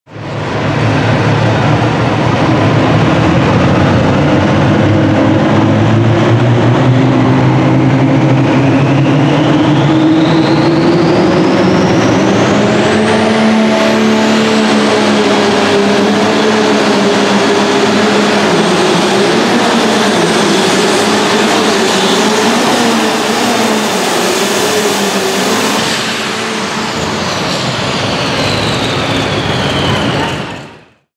Lawrence Phillips' Lil Insane | Southern Invitational | Let's Go Pulling & Thurston Pulling Photos
Lil' Insane
Lawrence Phillips
Light Limited Super Stock Tractors

2023 Southern Invitational
Murfreesboro, TN

Trackside camera: Let's Go Pulling
GoPro camera: Thurston Pulling Photos